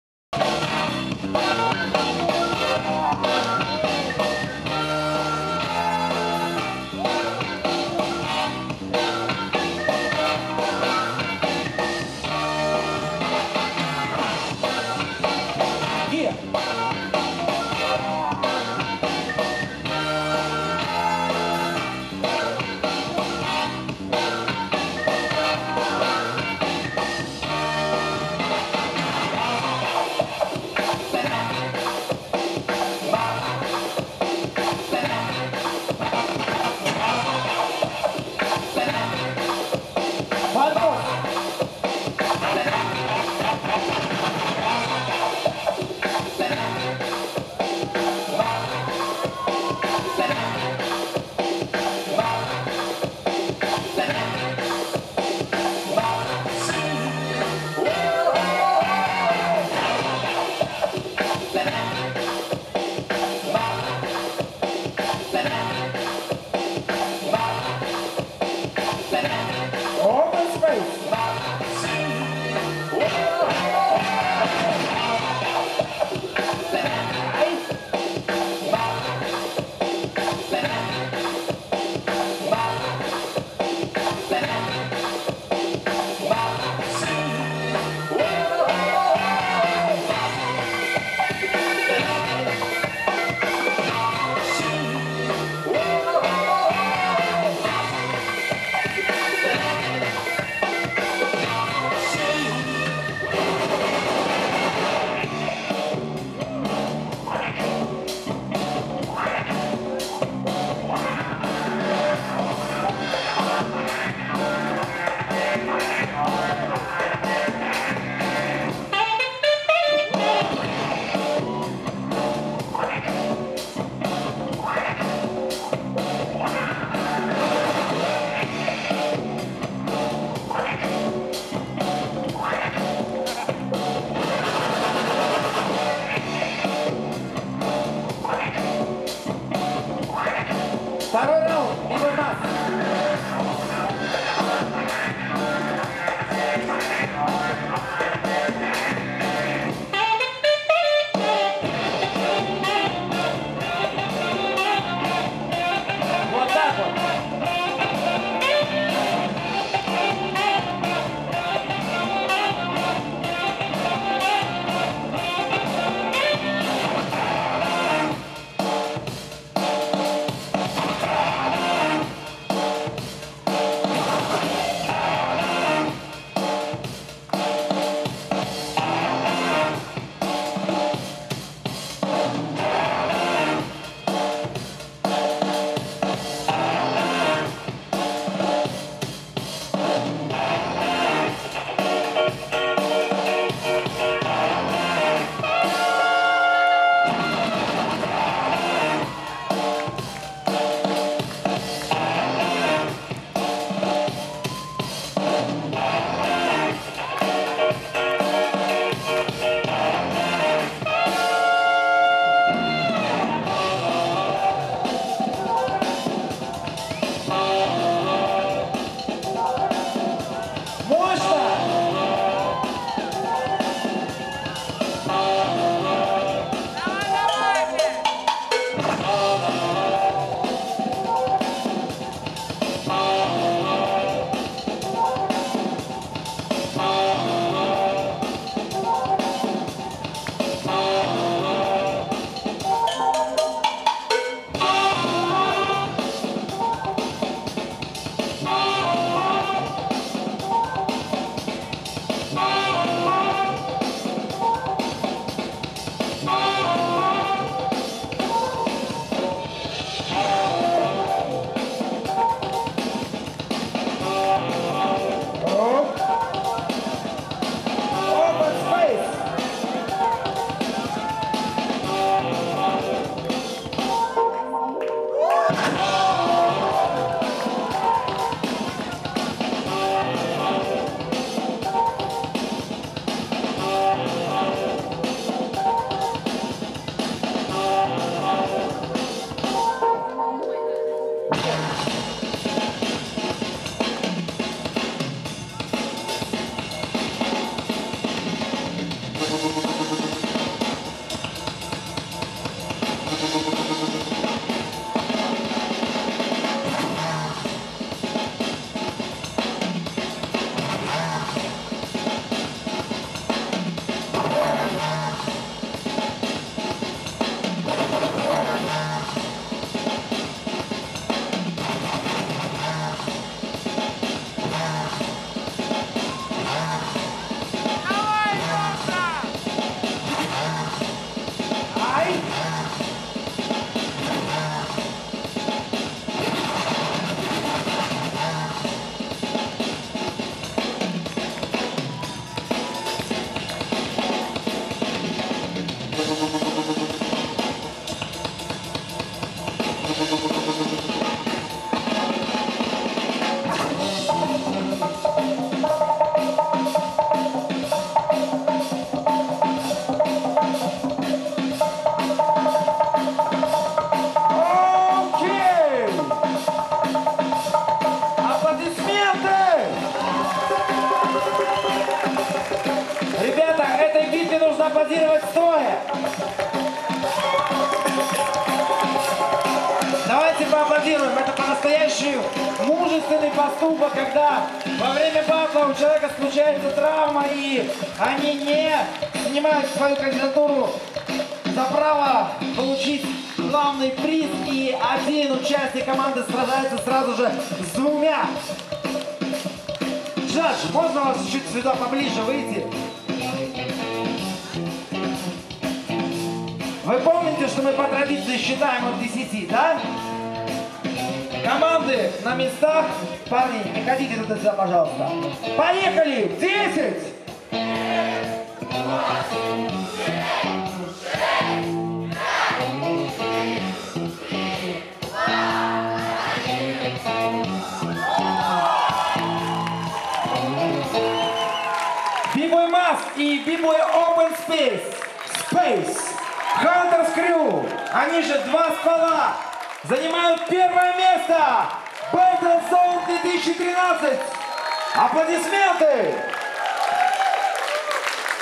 Battle zone 2013 | Breaking | Final 2stvola vs Hunters
bboys in Smolensk